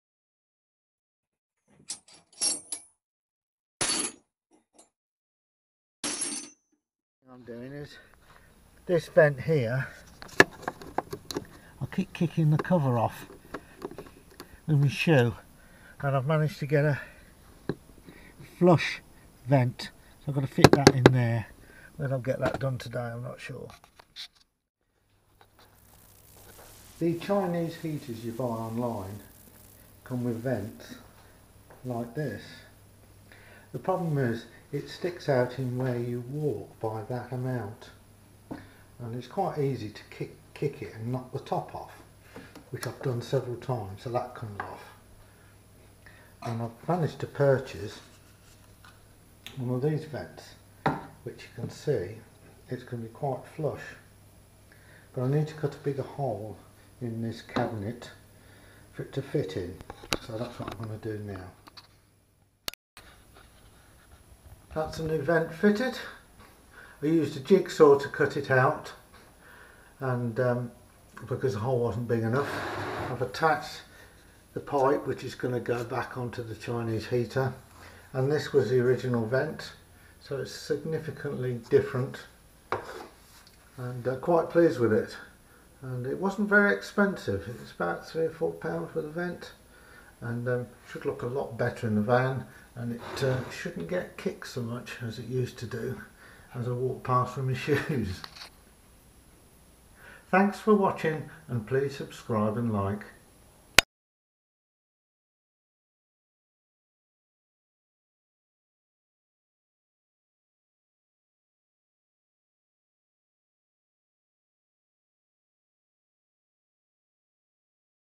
Chinese Diesel Heater Vent Upgrade
Fitting a replacement vent. Herb our Motorhome is small and I keep knocking the vent with my shoes. Flush fitting one is the cure. Ebay item Search For Auto Car Heater Ducting 75mm Warm Air Vent Outlet For Eberspacher Webasto Propex